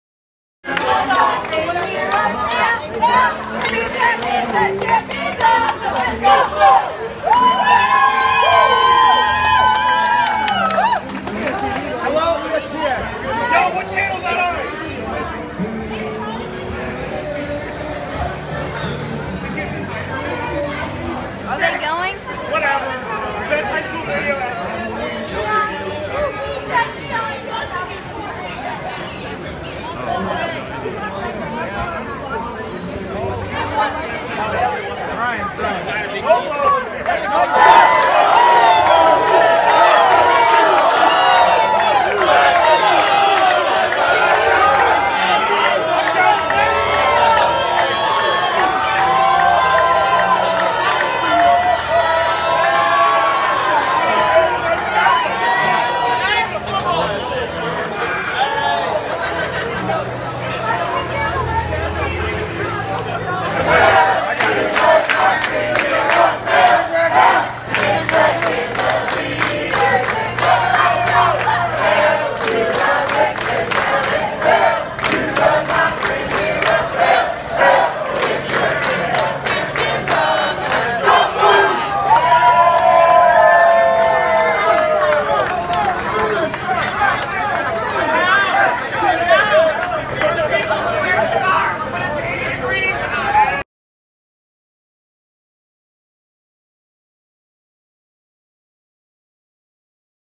Michigan basketball fans get rowdy on San Antonio Riverwalk
Michigan basketball fans prepare for Final Four game against Loyola-Chicago.